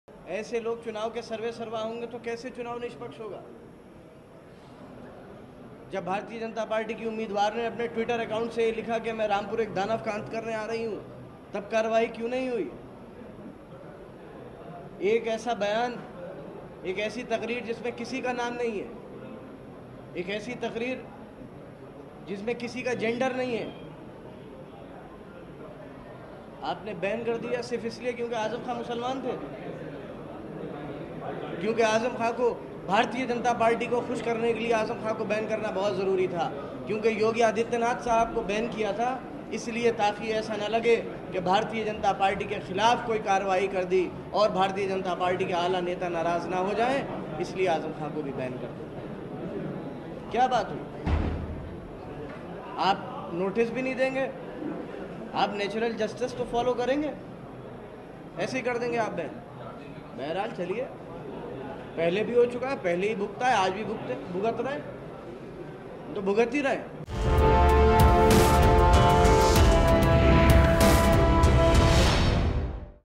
आज़म खान के बेटे ने चुनाव आयोग पर उठाए सवाल! | UP Tak
विवादास्पद बयान और आदर्श आचार संहिता उल्लंघन के मामले में बहुजन समाज पार्टी सुप्रीमो मायावती और समाजवादी पार्टी नेता आजम खान पर चुनाव आयोग द्वारा की गई कार्रवाई पर आजम खान के बेटे अब्दुल्ला आजम ने बयान दिया है. अब्दुल्ला ने कहा कि सत्ता में बैठे बीजेपी के लोग दलित और मुस्लिमों की आवाज को दबाना चाहते हैं